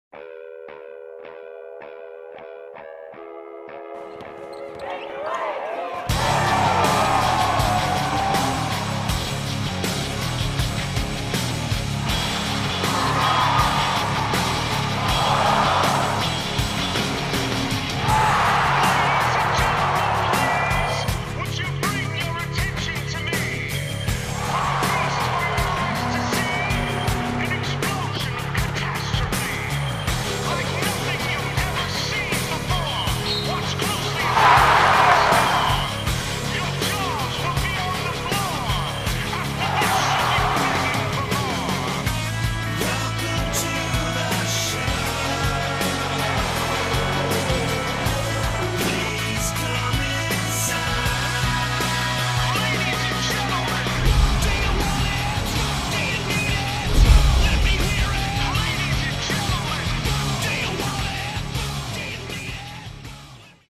2018 America East Championship Promo - (1) Vermont vs. (2) UMBC
The Cats and Retrievers square off for the America East title Saturday morning on ESPN2.